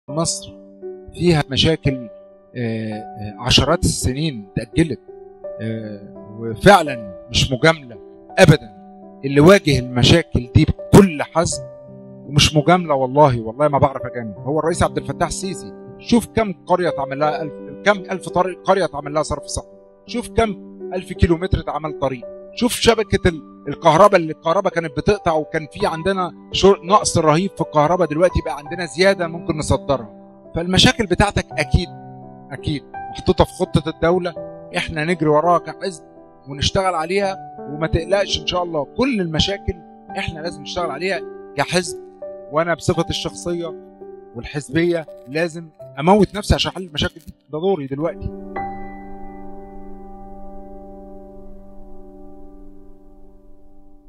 كلمه أحمد أبو هشيمة عن مجهودات الرئيس عبد الفتاح السيسي في تنمية مصر
هو أحد رجال الأعمال المصريين، رئيس مجلس إدارة مجموعة حديد المصريين، تأسست مجموعة حديد المصريين عام 2010، نجحت المجموعة في اختراق صناعة الحديد في مصر، تعمل المجموعة بفلسفة الإنتاج الآمن والجودة العالية للمنتجات التي تتطابق مع المقاييس المحلية والعالمية وذلك بإستخدام أحدث تكنولوجيا صديقة للبيئة مع مراعاة الحفاظ علي الموارد على المدى الطويل، تتكون المجموعة القابضة من ثلاث شركات كبرى وهي: شركة بورسعيد الوطنية للصلب و‌شركة أي أي سي لإدارة مصانع الصلب و‌شركة حديد المصريين لتجارة مواد البناء.